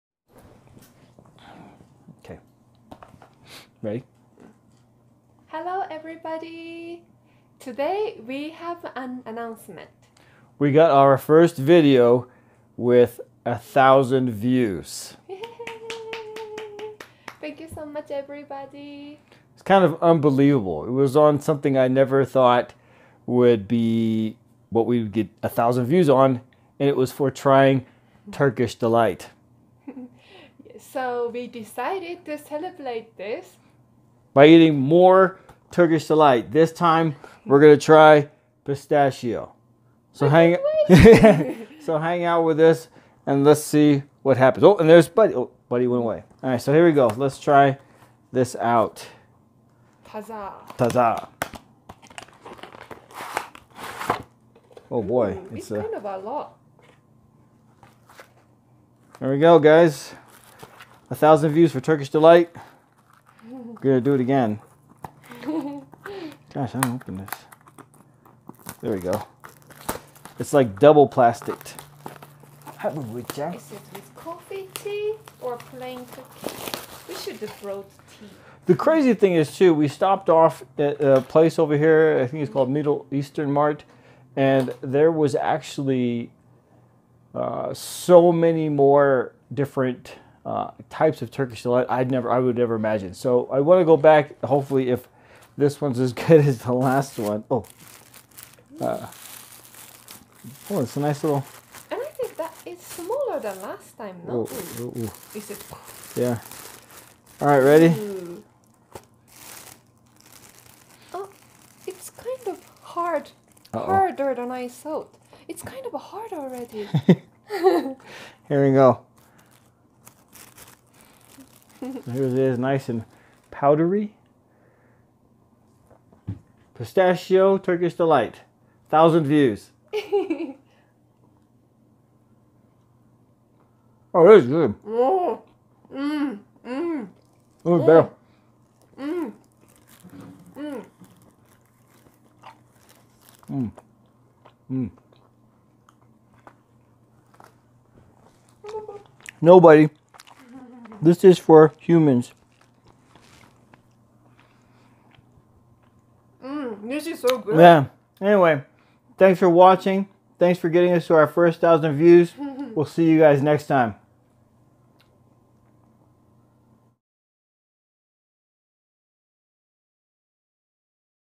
Celebrating our 1st 1K video (w/Turkish Delight)
Rodney and Rika celebrate their 1st video reaching 1,000 views. Thank you to all who have stopped by to watch our videos.